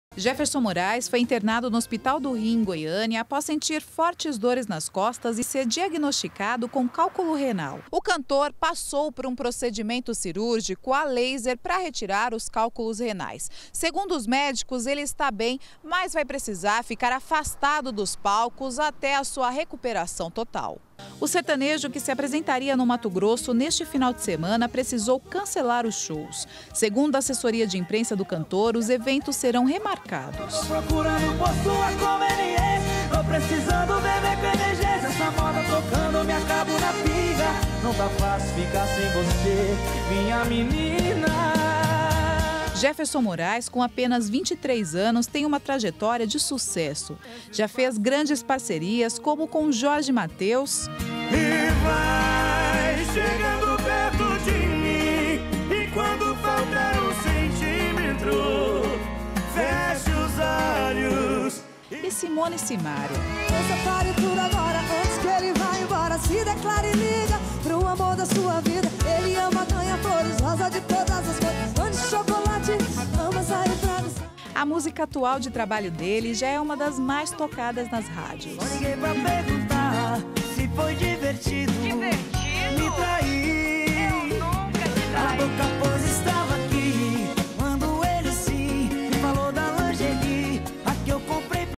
GA - Cantor Jefferson Moraes passa por cirurgia - 27-09-2018
O cantor Jefferson Moraes teve que passar por uma cirurgia de urgência aqui em Goiânia. Ele vai ficar afastado dos palcos ate se recuperar.